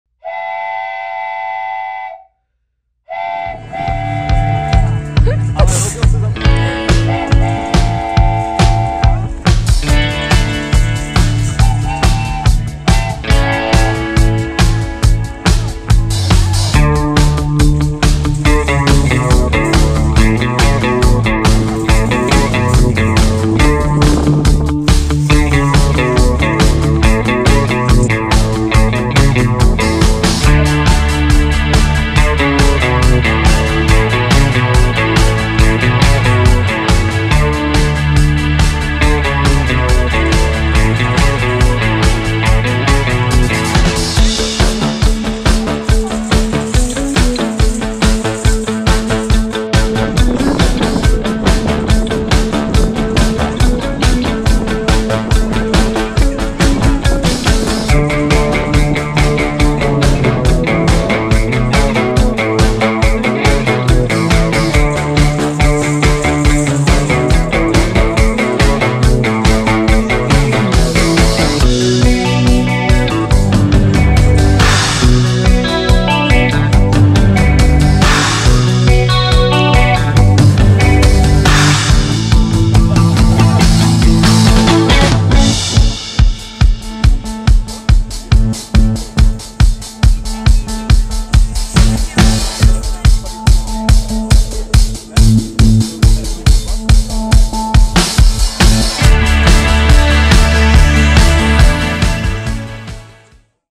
Nástupiště 9 3/4
Při příjezdu děti prošly zdí na nástupiště 9 a 3/4, odkud je náš Čarodějný expres odvezl do školy Čar a kouzel.